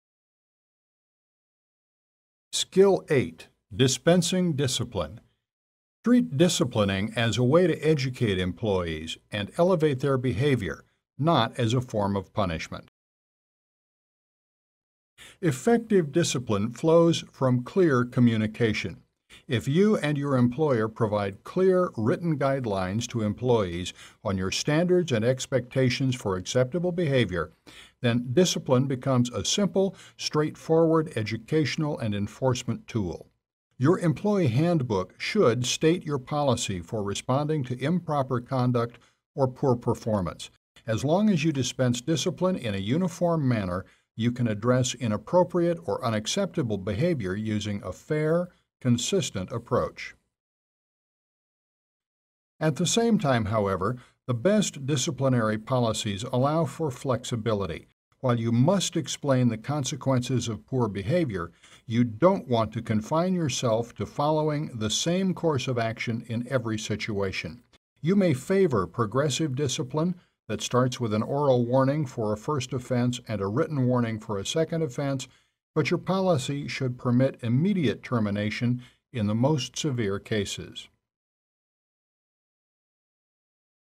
How to Discipline an Employee: How-to Use Discipline to Help an Employee Improve Productivity
- Rule when considering how to discipline an employee is to realize that discipline is not punishment and it is not a way to retaliate against your employees so you can finally "get them".